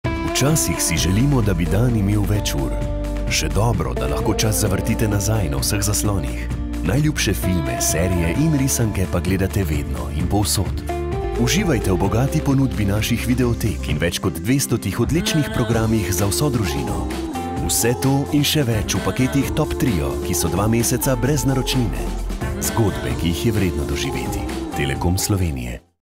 Najboljši filmi in serije v paketih TopTrio
Spremljajte najboljše filme, serije in risanke vedno in povsod. Več kot 1000 filmov v videoteki DKino in več kot 200 programov za vso družino. Vse to v paketih TopTrio, v akciji 2 meseca brez naročnine.